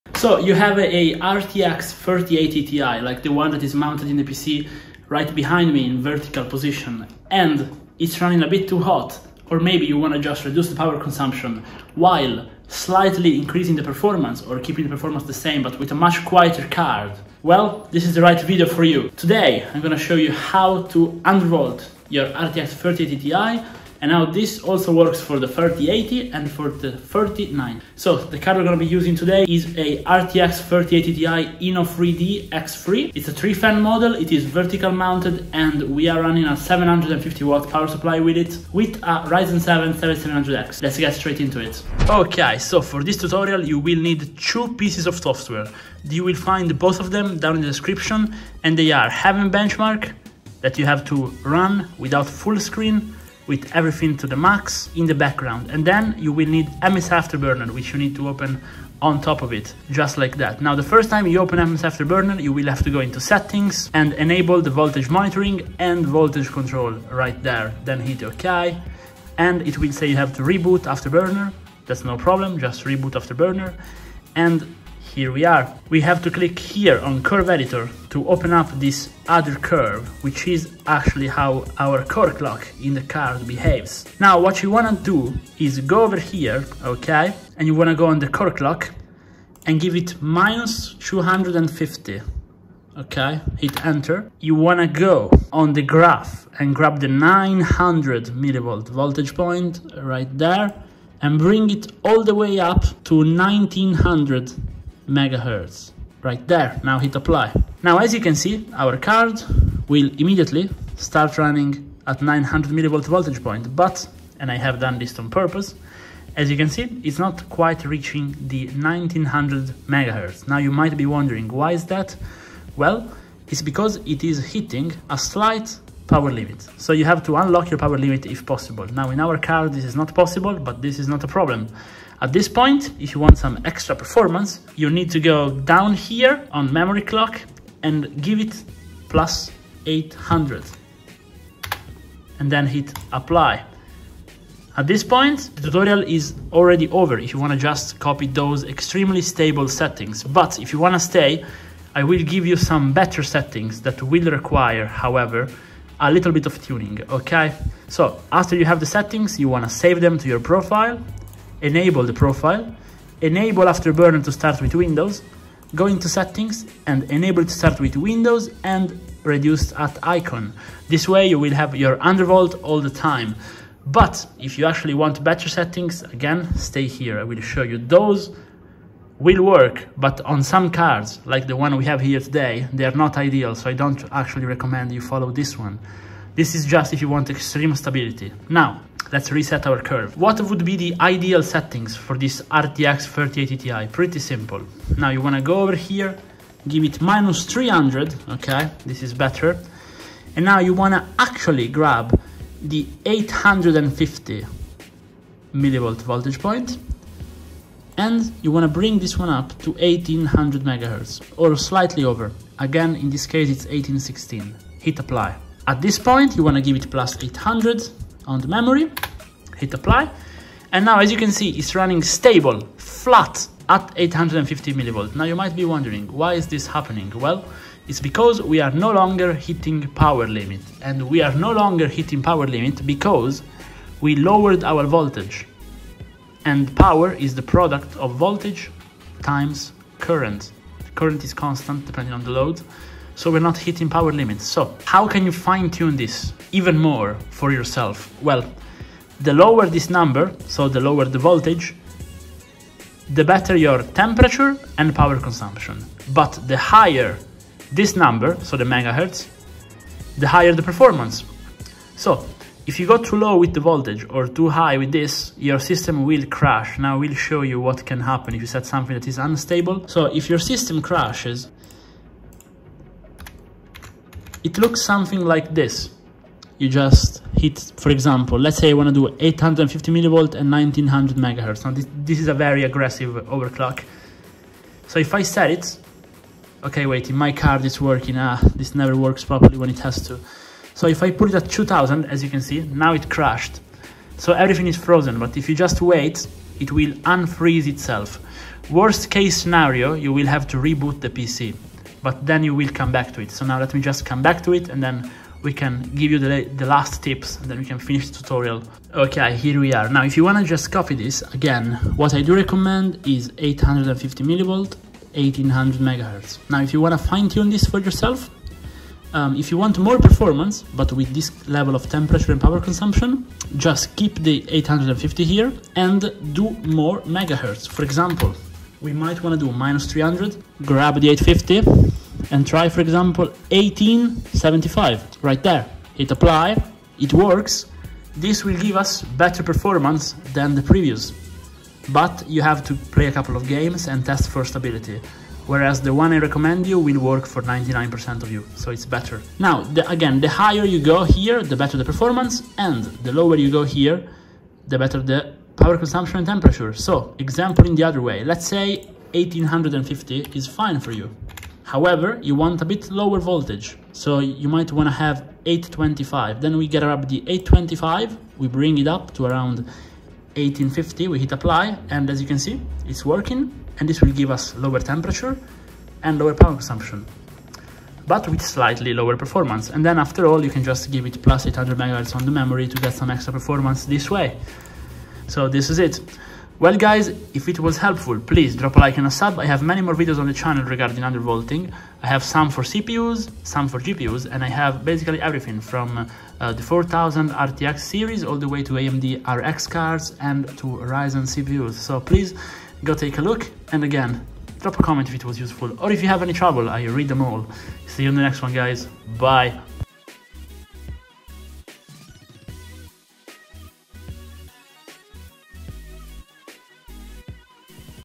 Undervolt your RTX 3080 Ti for more FPS and Lower Temperature! - Tutorial
Undervolting really seems to be the best option in terms of GPU optimization for the RTX 3000 series. Be it a 3090 Ti, 3080, 3070 Ti or even a 3060 Ti, I’d still recommend doing it. If you do it properly, you’re gonna lose virtually 0 performance or even get an increase, while dramatically reducing heat and power consumption. Today we are featuring a RTX 3090 PNY XLR8 triple fan and using an 850W 80 Plus Platinum Corsair PSU!

The tutorial also works for the RTX 3090 Ti

The software in the background is Heaven Benchmark, here's the